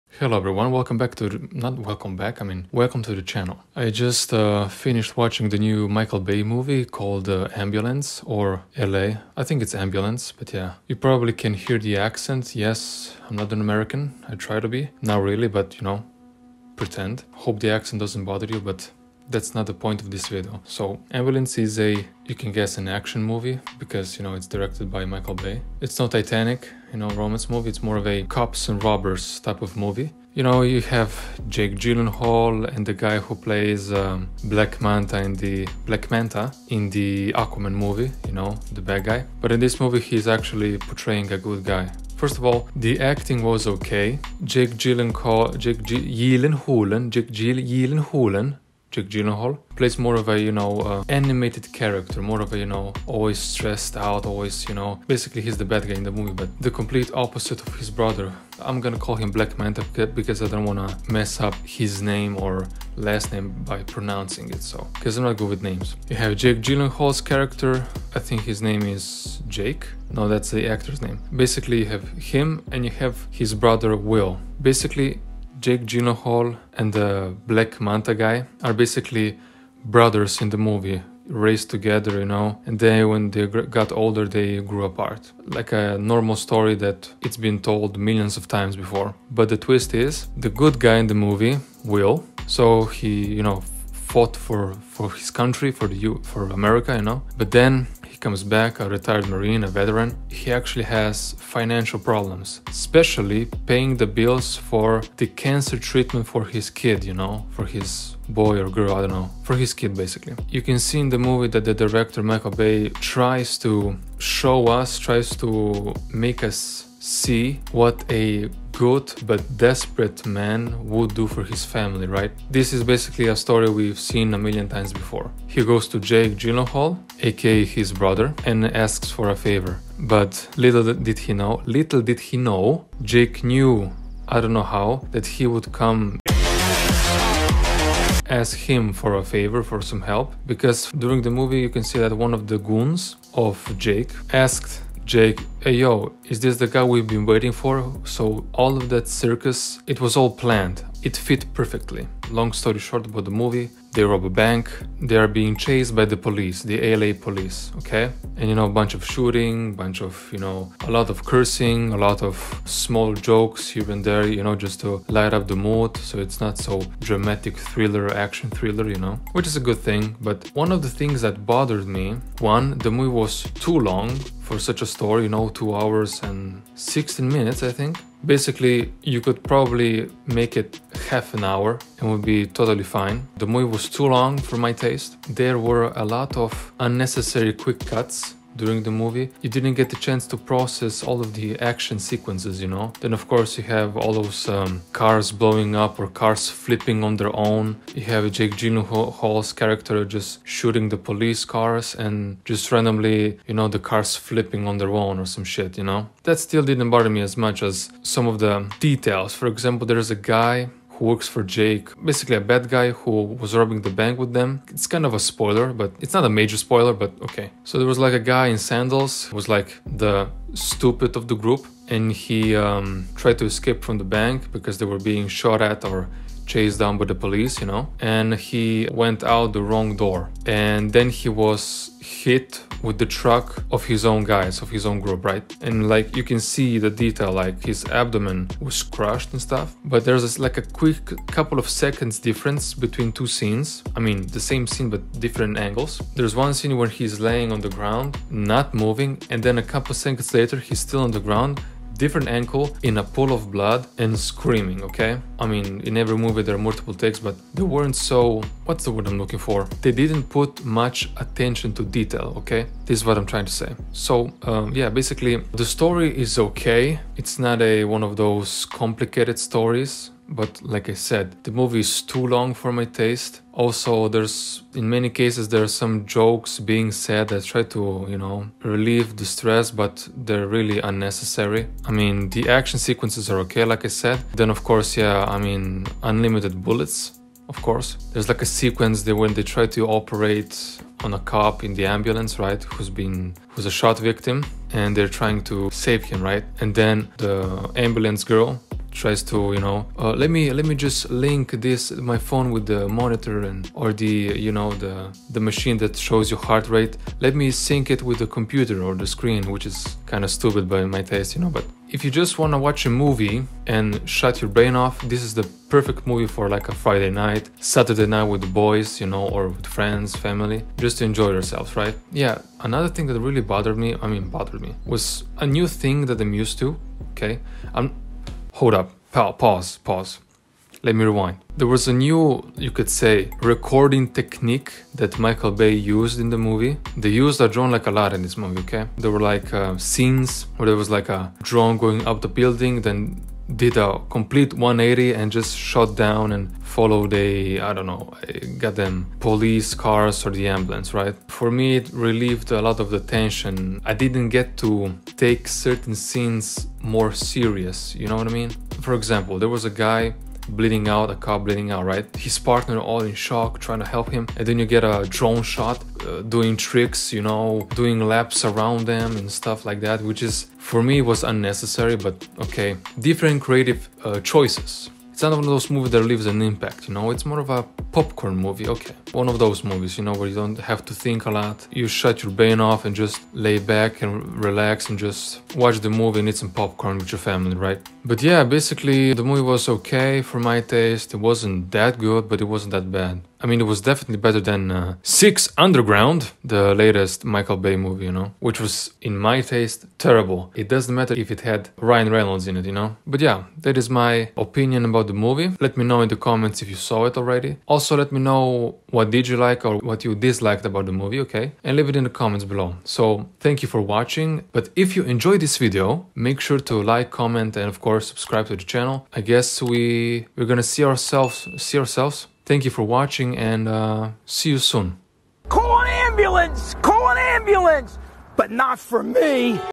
Ambulance (2022) | Honest Opinion
A movie in which Mysterio and Black Manta rob a bank and then steal an ambulance.